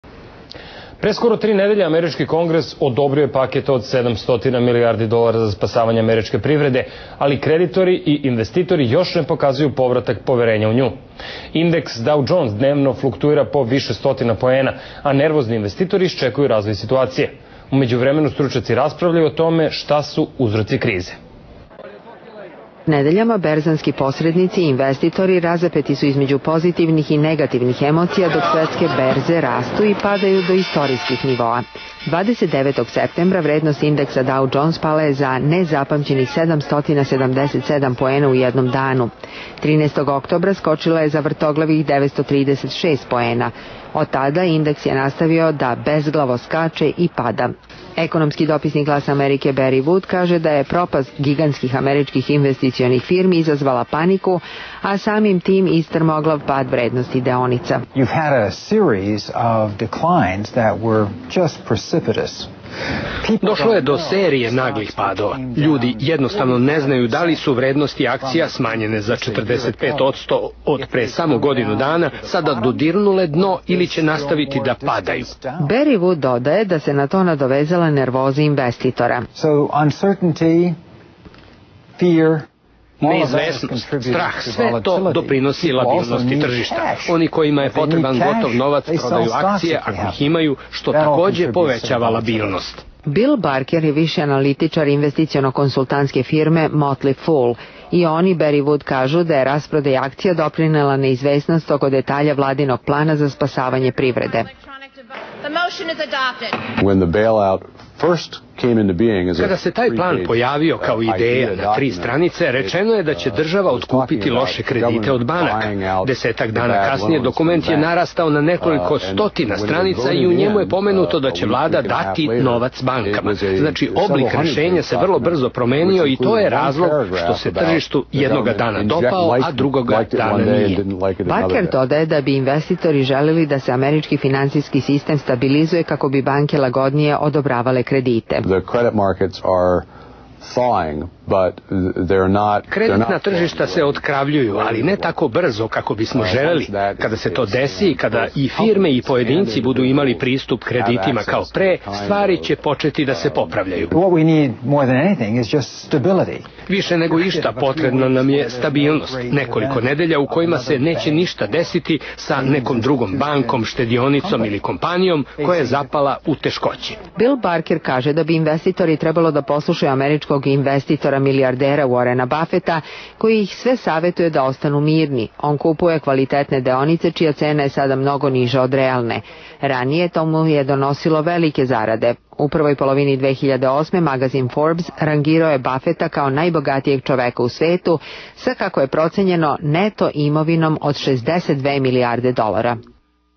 Nervoza investitora pospešuje finansijsku krizu u SAD
Pre skoro tri nedelje američki Kongres odobrio je paket od 700 milijardi dolara za spasavanje američke privrede, ali kreditori i investitori još ne pokazuju povratak poverenja u nju.